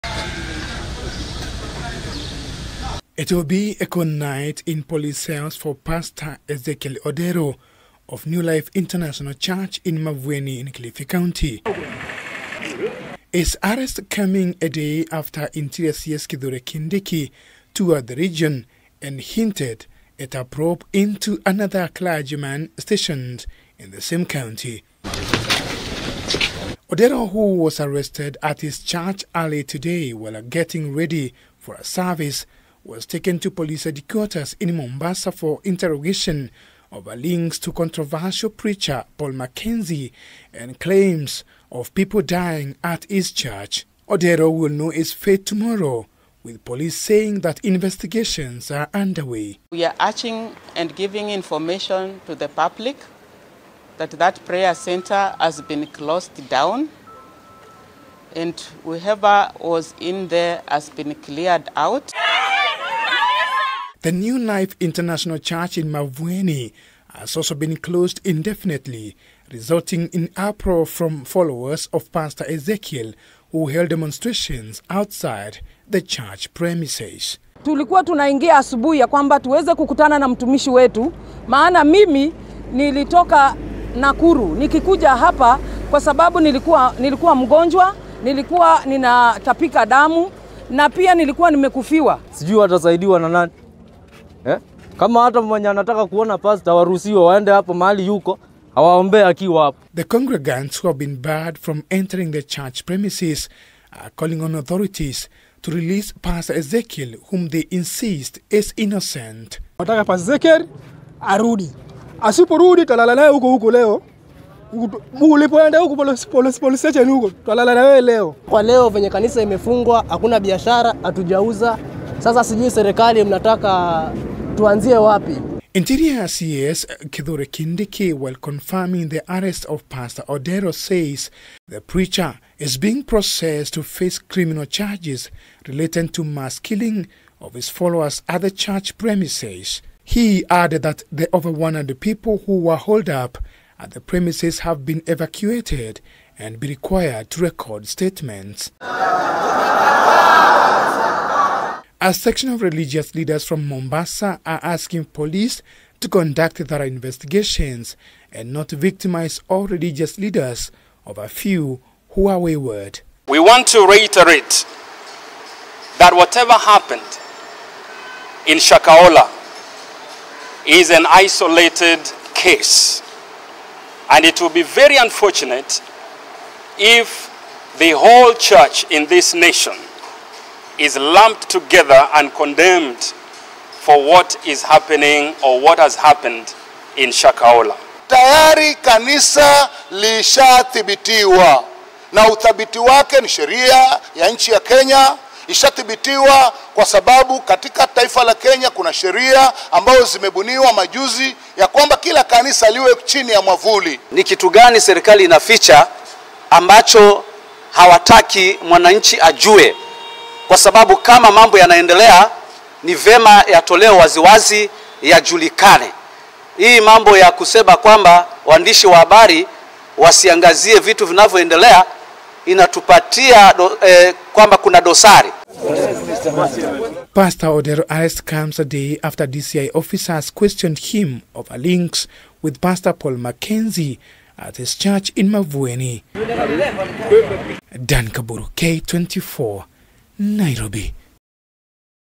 New Life Pastor, Ezekiel Ombok Odero is in police custody
Pastor Ezekiel Ombok Odero will cool his heels in police cells tonight as police expand the probe into Shakahola deaths and potential religious crimes taking place in other parts of the region. Odero was arrested earlier today at his church premises while preparing for a service. Followers of pastor Ezekiel Odera have since held demonstrations demanding his release.